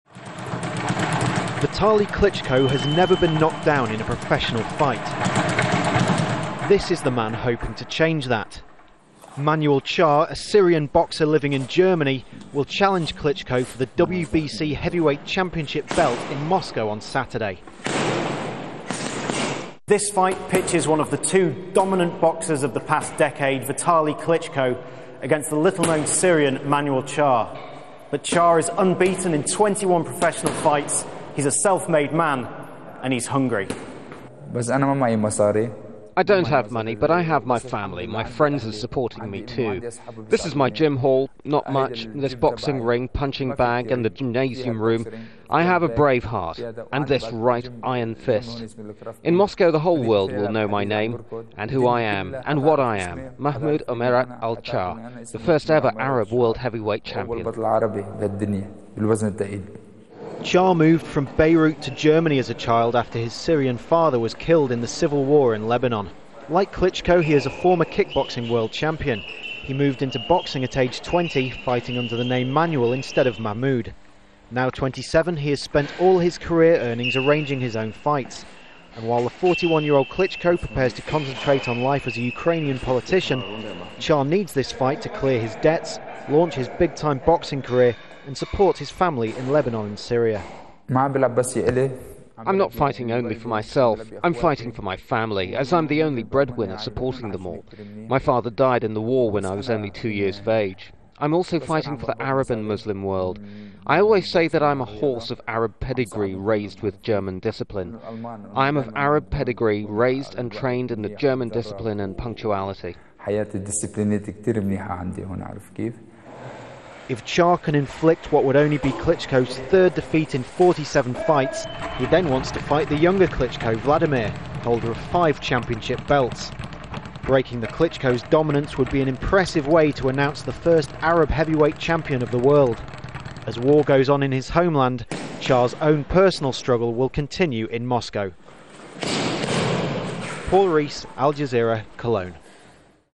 Arab heavyweight boxer to face Klitschko
A boxer from Syria is attempting to become the first Arab heavyweight champion of the world.  Manuel Charr will fight Vitali Klitschko for the WBC title on Saturday in Moscow.  It could be one of Klitschko's last fights before concentrating on his political career.   But for the Syrian, the stakes are much higher.  Al Jazeera's Paul Rhys met Charr at his training base in Germany.